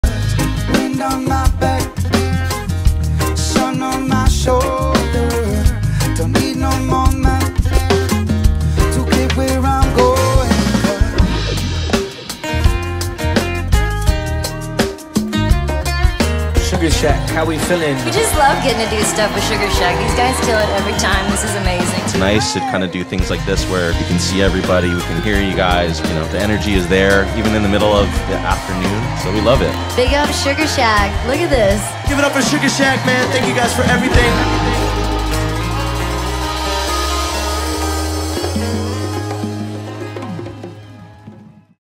Sugarshack Pop-Up x Wonderfront Announcement
We’re bringing our pop-ups to San Diego, California!

Come catch us at Wonderfest Music Festival November 18-20, 2022

Swing by the Sugarshack stage for Live Music sets & podcasts with their amazing lineup of artists (plus some SoCal locals!)

It’s going to be an epic time and we can’t wait to see y’all there! 🤙